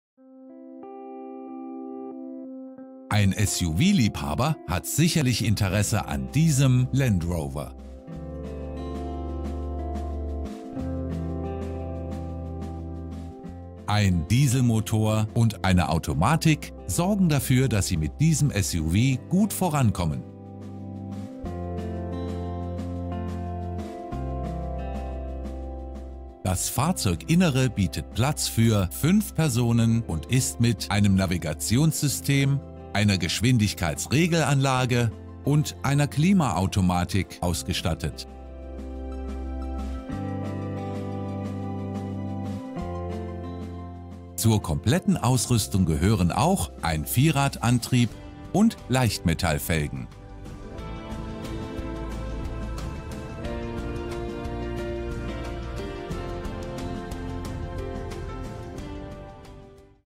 Land Rover Discovery Sport
Verbrauch komb: 5,3 l/100km
Verbrauch innerorts: 6,3 l/100km
Verbrauch außerorts: 4,7 l/100km
CO2 Emmision komb.: 139,0 g/km
CO2-Effizienz: D

Hersteller: Land Rover
Modellvariante: Discovery Sport
Leistung: 150 kW
Hubraum: 1.999 cm3
Kraftstoff: Diesel
Schadstoffklasse: euro 6
Getriebe: Automatik
Gesamtgewicht: 2.510 kg
Karosserie: all-terrain vehicle (5 drs)
Farbe: anthrazit

Ausstattung:
* Fahrer-/Beifahrer-Airbag)
* Polsterstoff
* ABS (Antiblockiersystem)
* Servolenkung
* Zentralverriegelung
* Klimaautomatik
* Allrad
* LM-Felgen
* Navigationssystem
* Elektrische Fensterheber
* Tempomat
* Traktionskontrolle
* Elektrische Wegfahrsperre
* Metallic
* ESP (Elektronisches Stabilitäts-Programm)
* Kindersitzvorbereitung (ISOFIX)
* Regensensor
* Multifunktionslenkrad
* Partikelfilter
* Bluetooth
* Start-/Stopp-Automatik
* Freisprecheinrichtung
* Lichtautomatik
* Einparkhilfe hinten
* Garantie
Beschreibung: